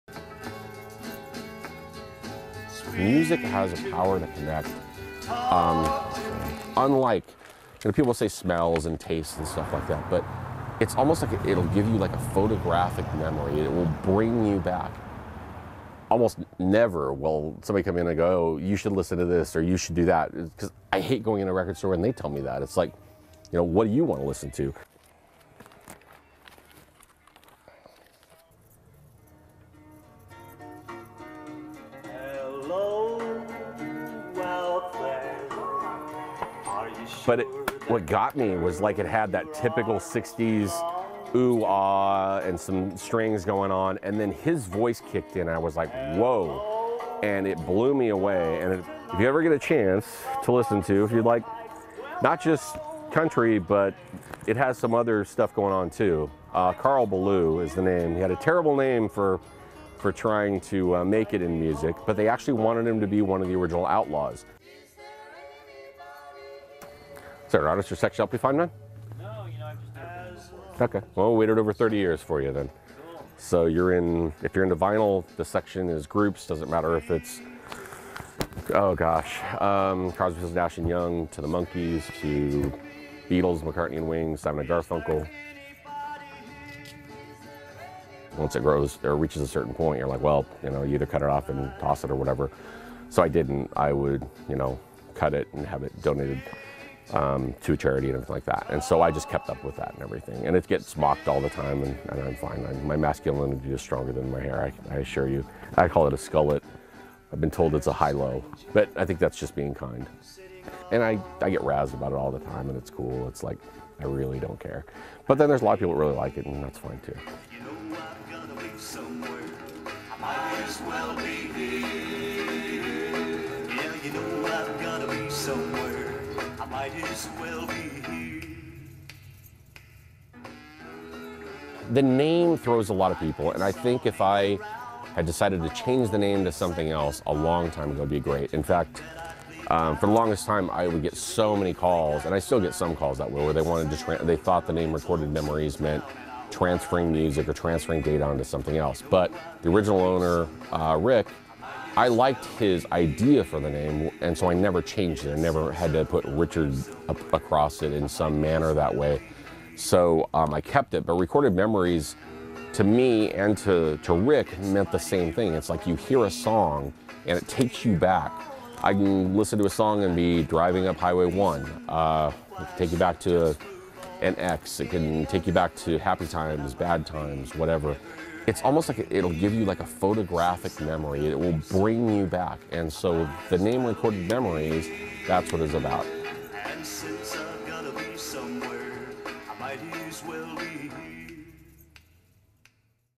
KREM Short Stories: Richard, Hamilton Street
Richard Terzieff spoke with KREM 2 photojournalist Nathan Brand about the power of a record as part of the weekly series "KREM Short Stories." He said, "It’s like you hear a song and it takes you back.  I can listen to a song and be driving up Highway 1.  It can take you back to an ex.  It can take you back to happy times, bad times, whatever."